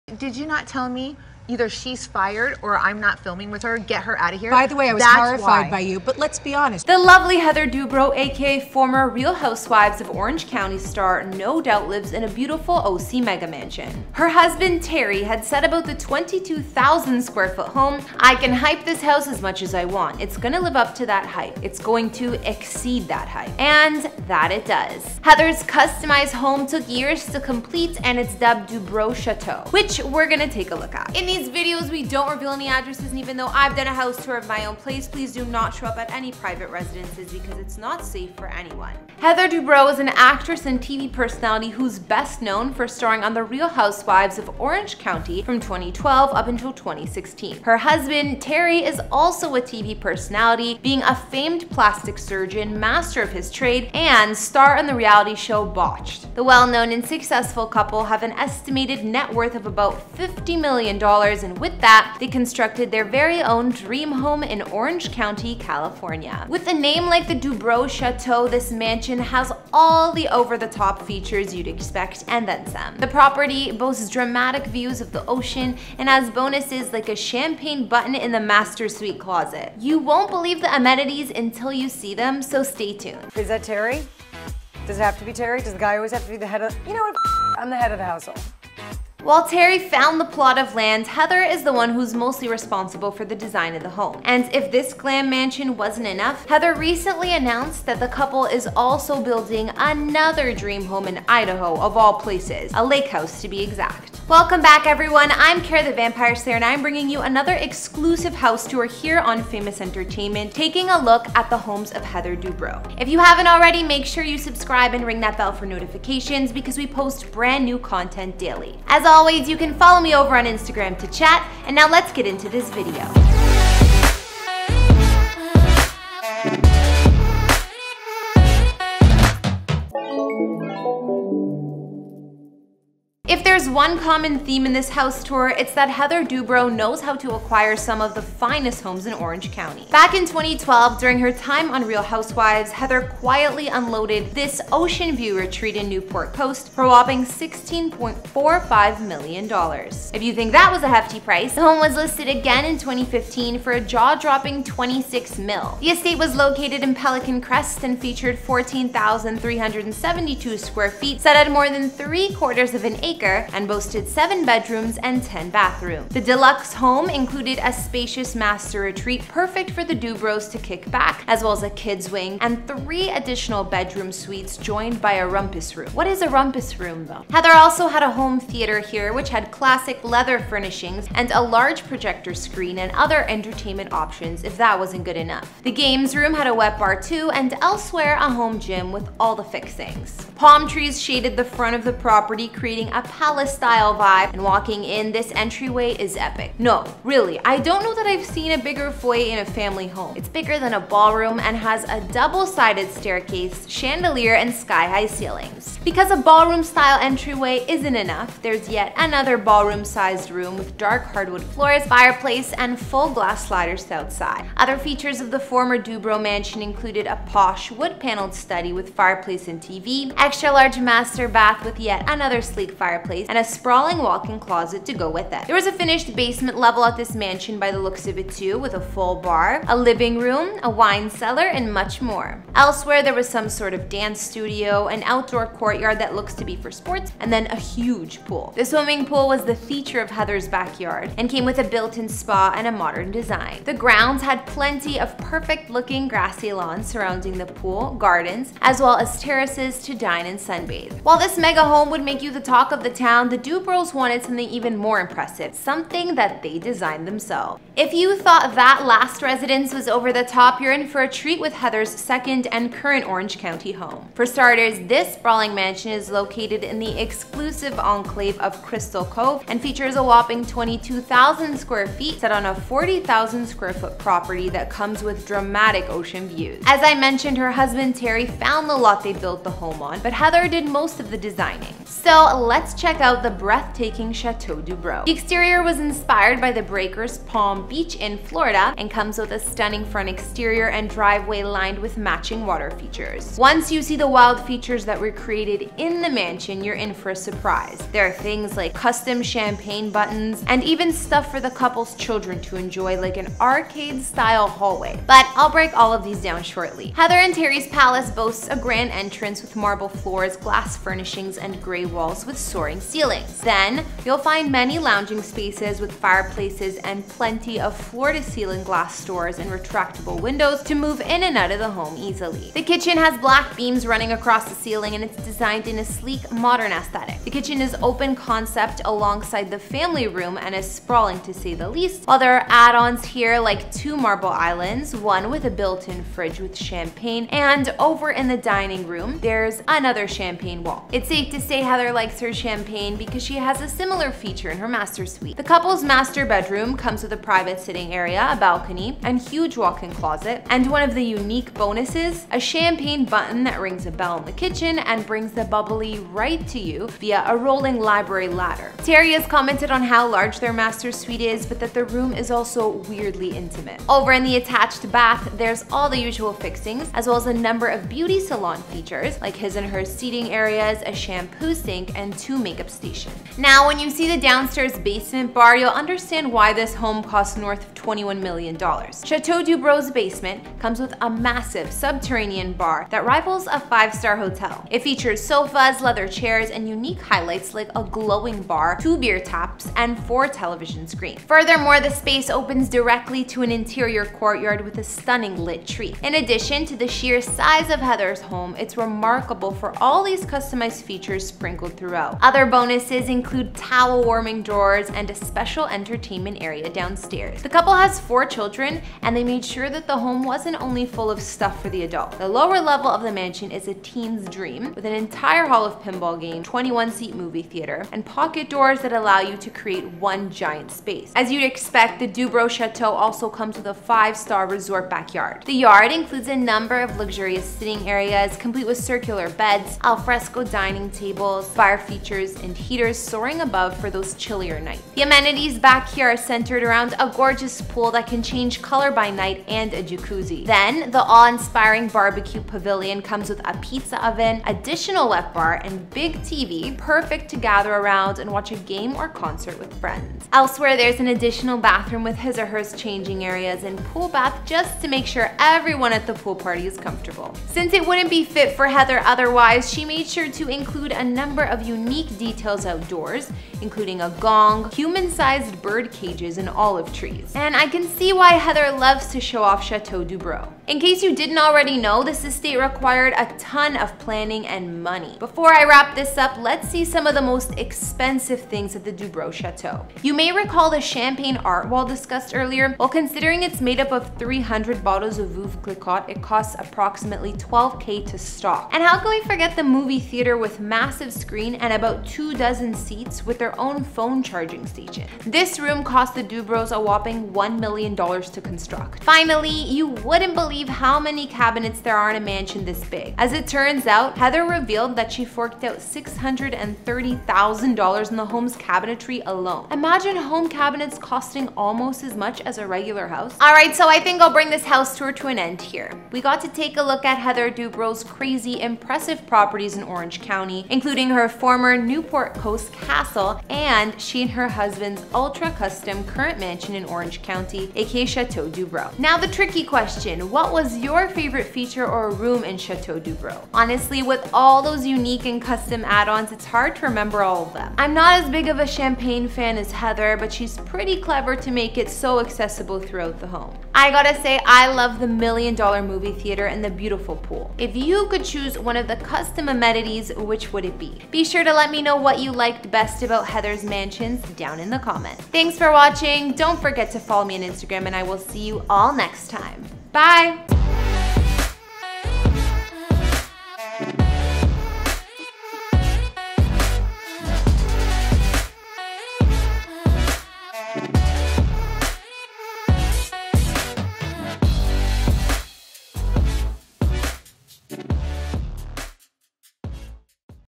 Heather Dubrow | House Tour | $21 Million Orange County Mansion & More
The lovely Heather Dubrow aka former Real Housewives of Orange County star no doubt lives in a beautiful Orange County mega-mansion. Her husband Terry had said about the 22,000 square foot home. “I can hype this house as much as I want. It’s going to live up to that hype. It’s going to exceed that hype,” And that it does. Heather’s customized home took years to complete and it’s dubbed “Dubrow Chateau” which we’re gonna take a look at.